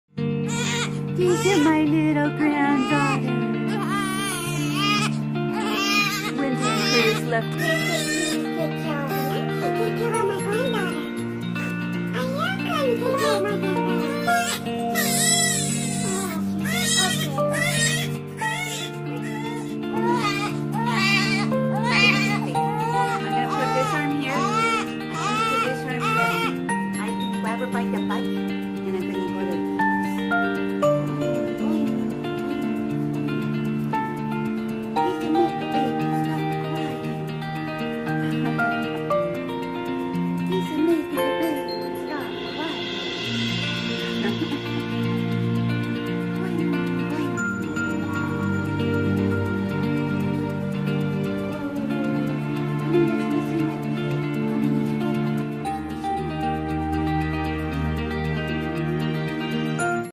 How to Make a Baby Stop Crying.
This is the sweet little crybaby. My most excellent granddaughter in the whole wide world ...Vivian! Viv, take a bow!